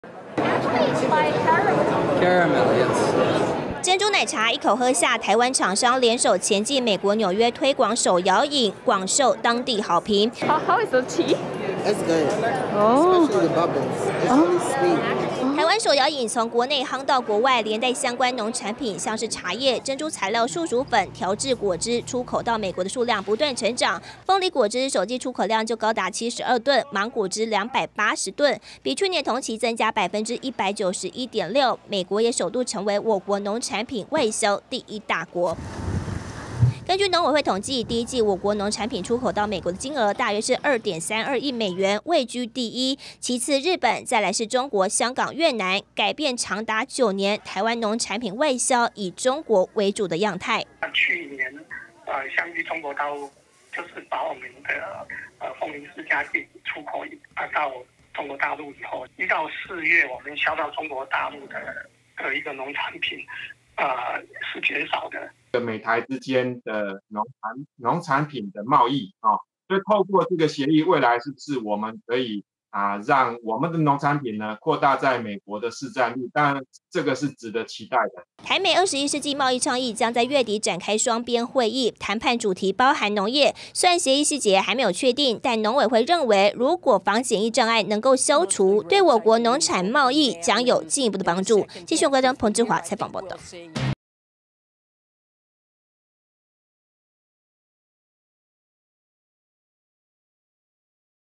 美躍台農產第一外銷國 台美貿易倡議望助攻｜鏡新聞
過去一年中國刻意封殺台灣農產品，造就了美國取代中國，成為台灣農產品外銷第一大出口國家。以今年第一季，台灣農產品外銷總量45萬公噸來看，美國就占了18.3%，其次則是日本。長達9年台灣農產品外銷以中國為主的樣態，也被改變，專家認為，月底首次展開雙邊會談的台美21世紀貿易倡議，有助台灣農產品提高在美國的市占率。